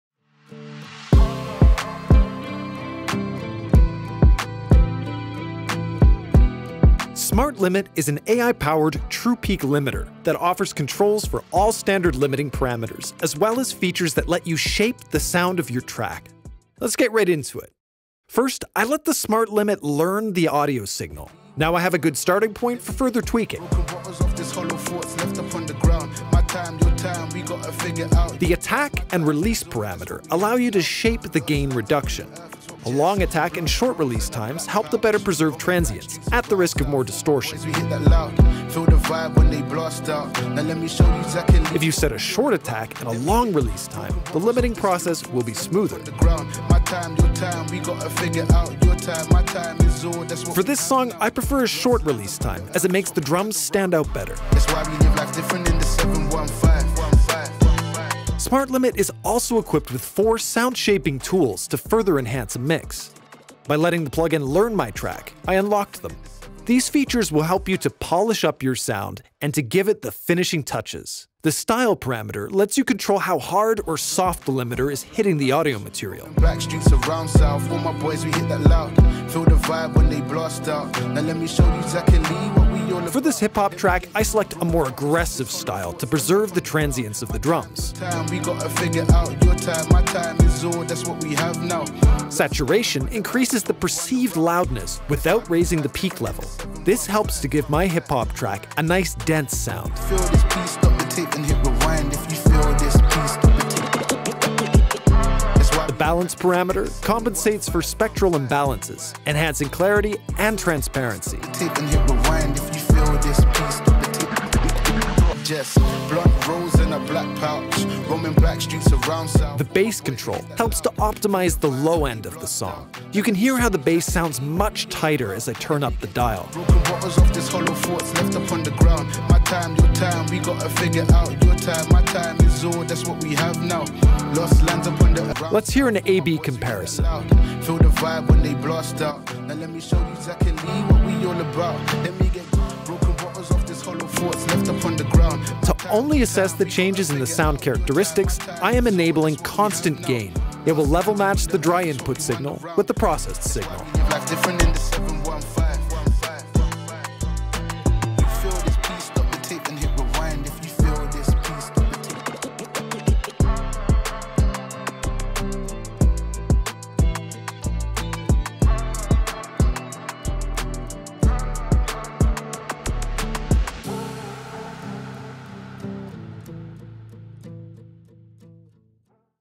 Fine-tuning your track with smart:limit | sonible
smart:limit offers four sound-shaping tools that allow you to shape your sound to perfection before its release.

Chapters:
0:00 Introducing the AI true peak limiter
0:17 Let smart:limit learn
0:28 Gain, attack and release in smart:limit
0:58 Sound shaping tools
2:25 Constant gain